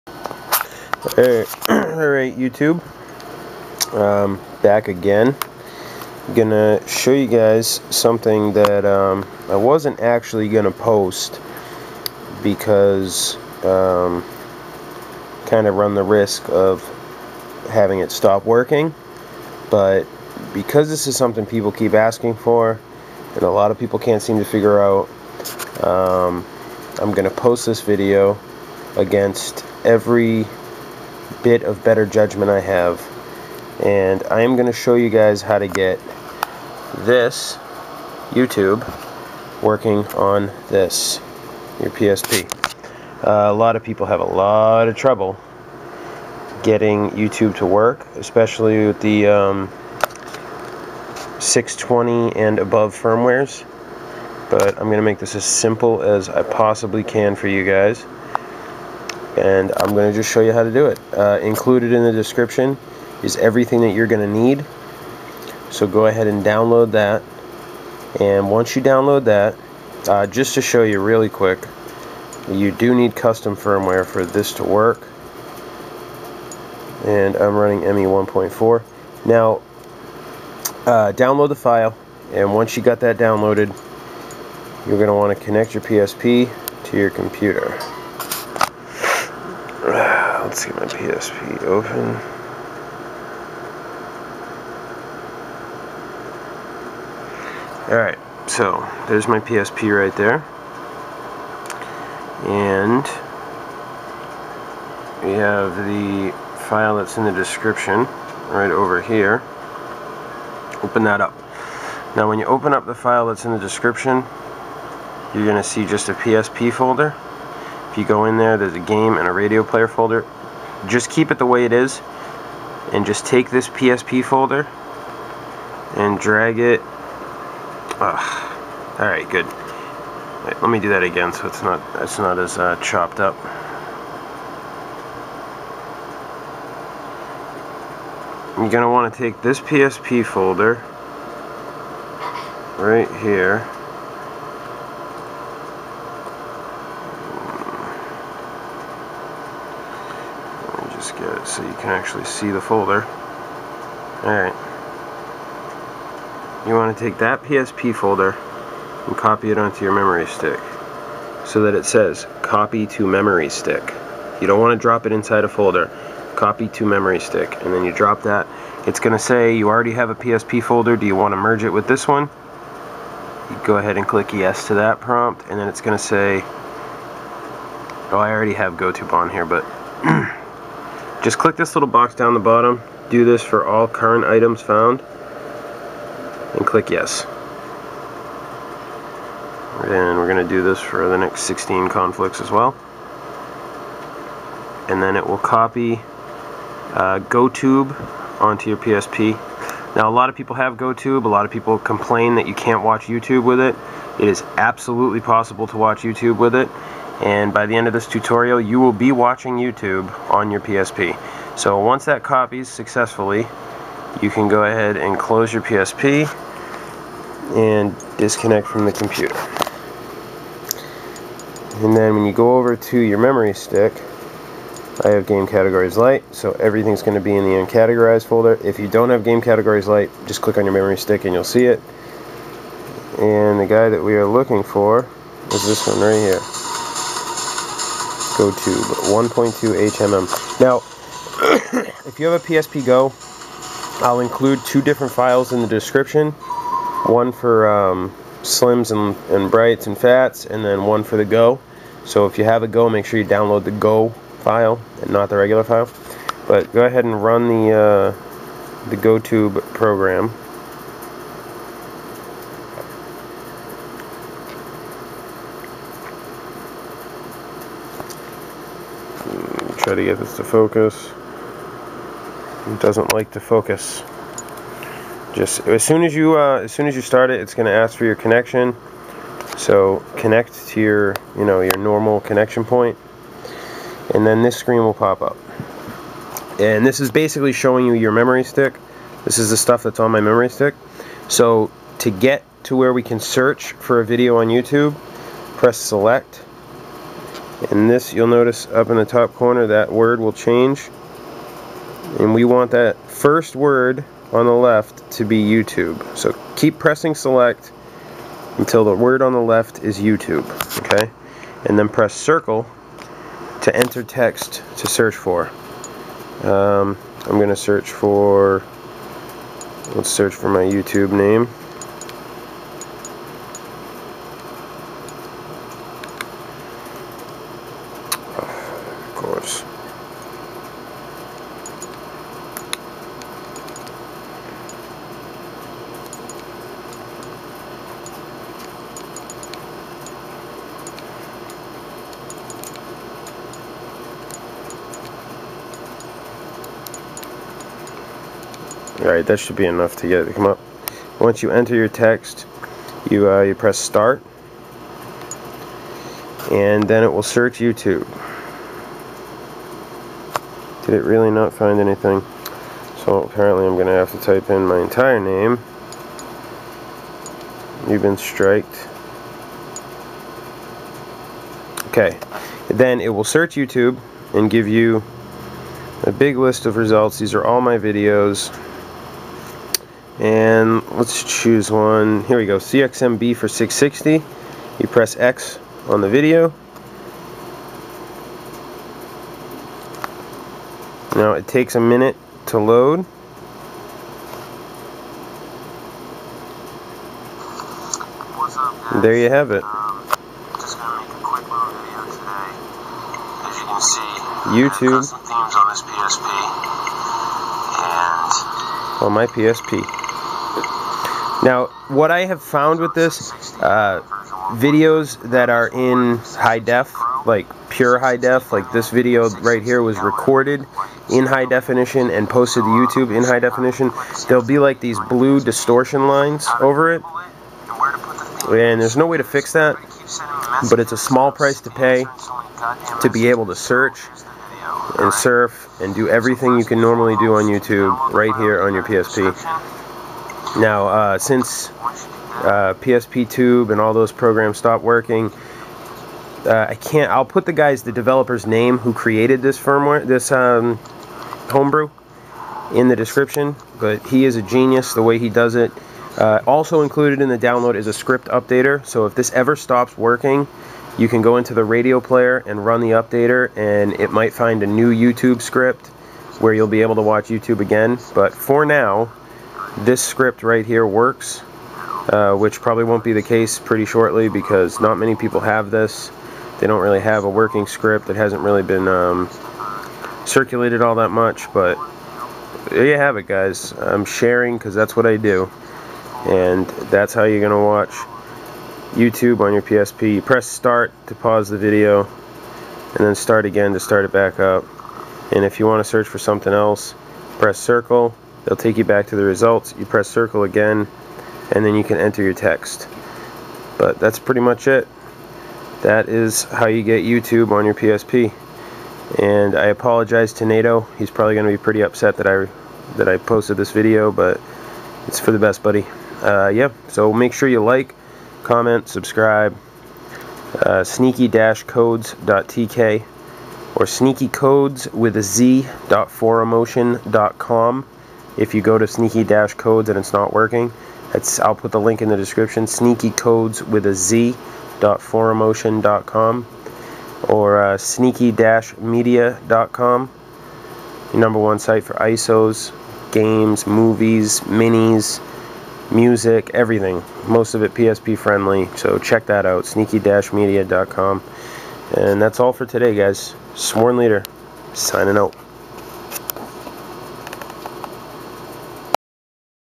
HOW TO WATCH YOUTUBE *LIVE* ON YOUR PSP WITH GOTUBE - STILL WORKING!! 3-29-12!!!
UPDATED 3/29/12
Fixed links, changed to Mediafire.  All links working 100%, just download the one that matches your PSP model.
UPDATED 3/19/12

Just a quick video to prove and show that it is indeed possible to watch YouTube on your PSP.  This is a homebrew application called GoTube 1.2 (the version in the video is the HMM High Memory Mod version), and it is BADASS.  YouTube is one of the ones that usually does not work, but with this version, there is a working YouTube script, to finally get some YouTube on your PSP :)

Just copy the PSP folder to the root of your memory stick, and click YES to all the prompts.  You can update the scripts through the RadioPlayer tab on your XMB, but you don't need to in order to use it, it works the way it is.

PSP Friendly site for Movies, Music, TV Shows, ISOs, Minis, and much more!

Credits:
Video By: SwoRNLeaDejZ
GoTube By: Biscottealacrevette
Epicness By: Biscottealacrevette

:P